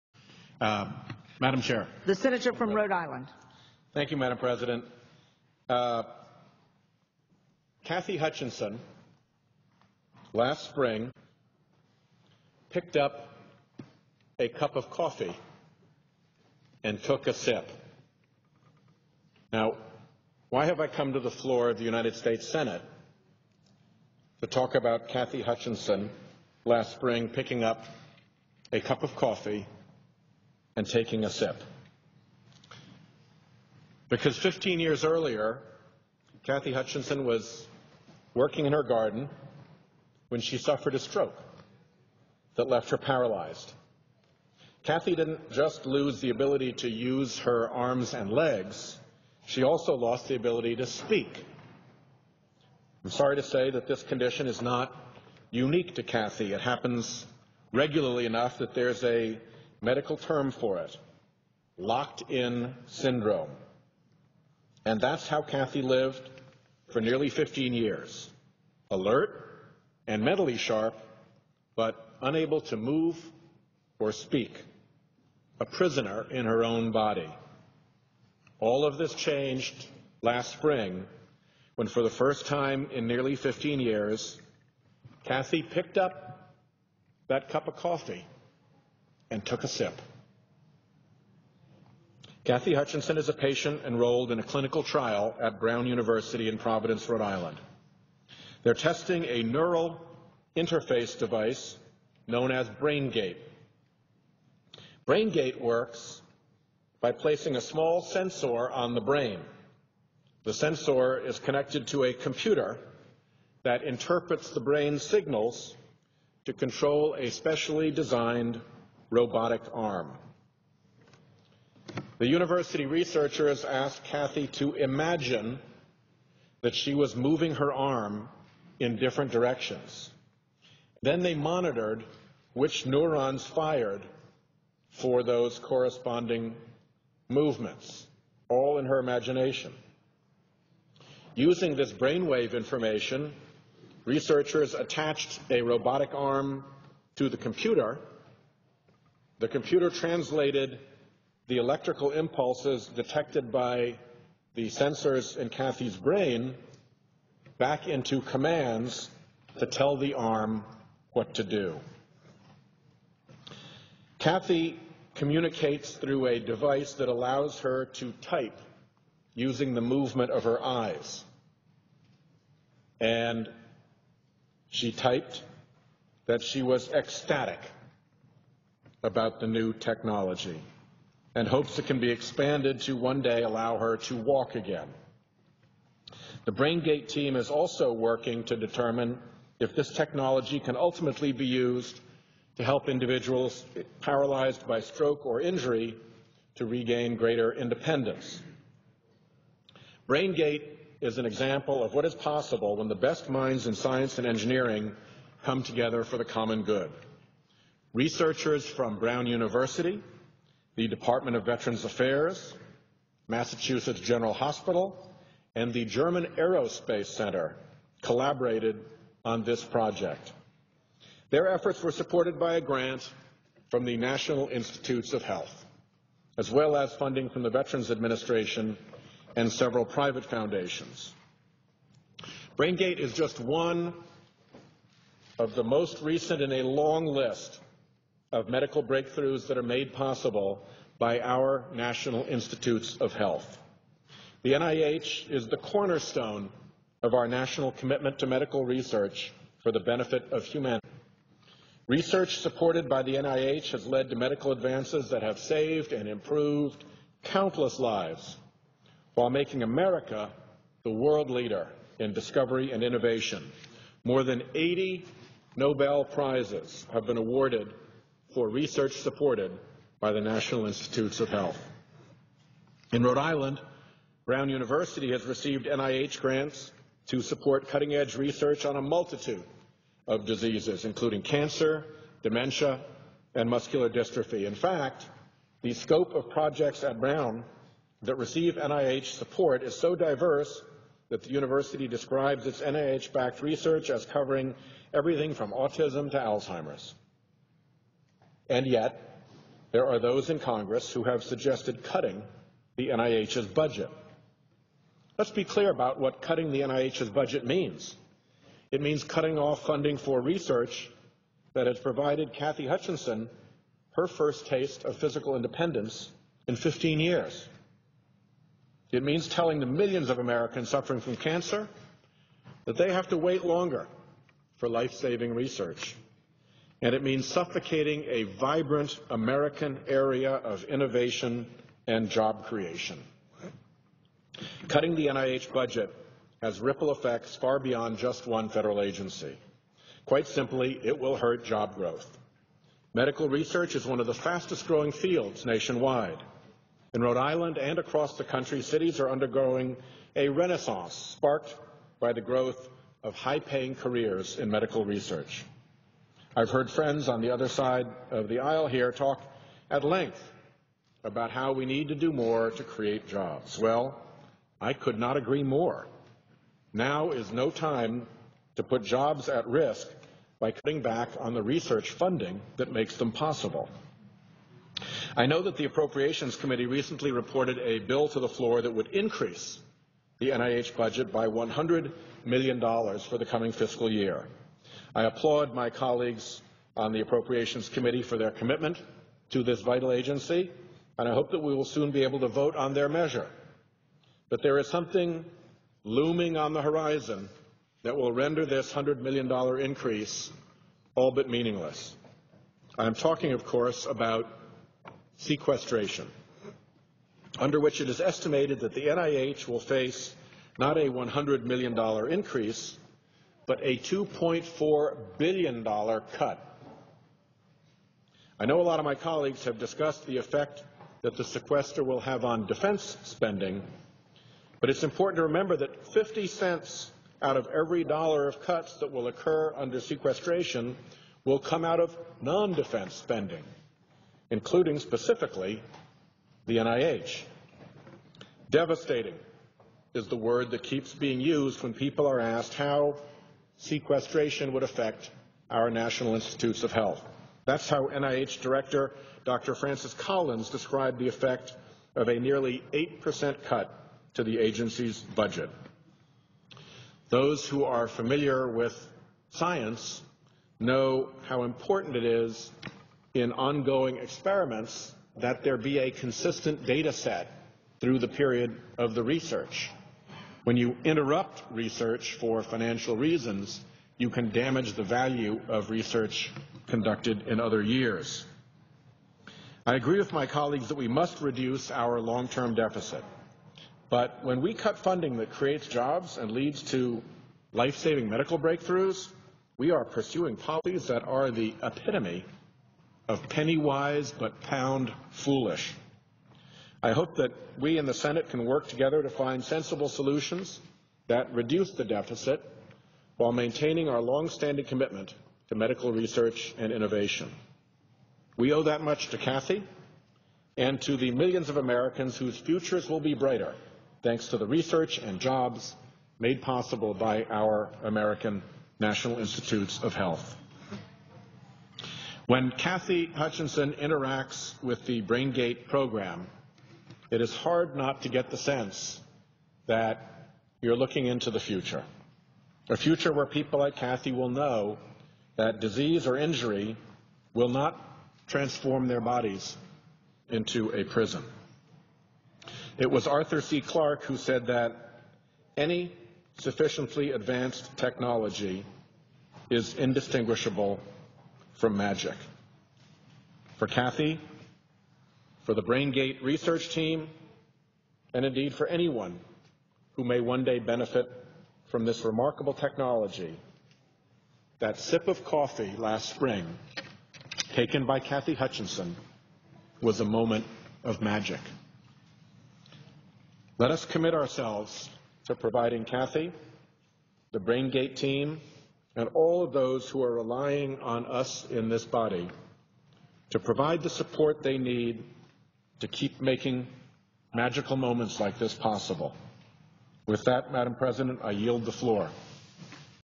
Sheldon Recognizes Brown University "BrainGate" Program
In this speech on the Senate floor, Sheldon praised the Brown University "BrainGate" research program that helped one paralyzed woman pick up a cup of coffee using a mechanical arm controlled by her brain.  He also called for preserving federal funding for the National Institutes of Health, which helped fund the BrainGate program.